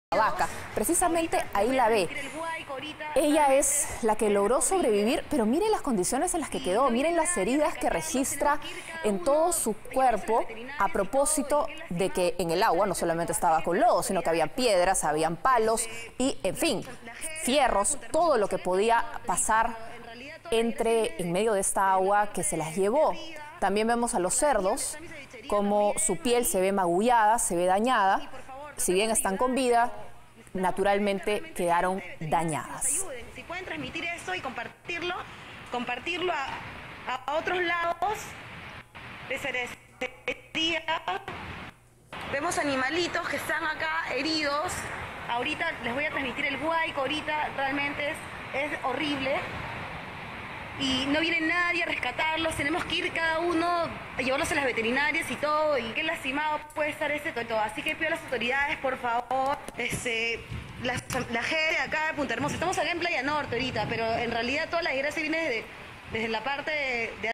Punta Hermosa: así quedó la vaca sobreviviente al huaico
Pobladores de Punta Hermosa auxiliaron a la vaca que sobrevivió a la fuerza del huaico.

Con grandes heridas en todo el cuerpo quedó esta vaca que sobrevivió a la fuerza del huaico que pasó por Punta Hermosa. De igual forma otros animales que se salvaron de un terrible final fueron auxiliados por los pobladores de la zona.